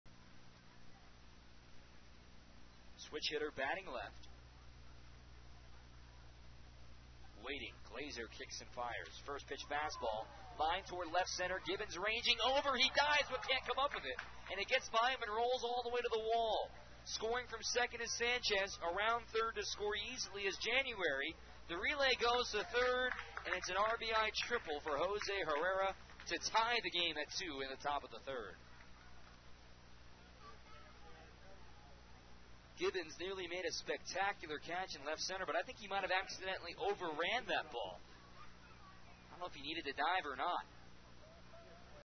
Herrera triples in two for Osprey
8/10/16: Missoula's Jose Herrera rips a two-run triple to left as the Osprey top Orem, 7-4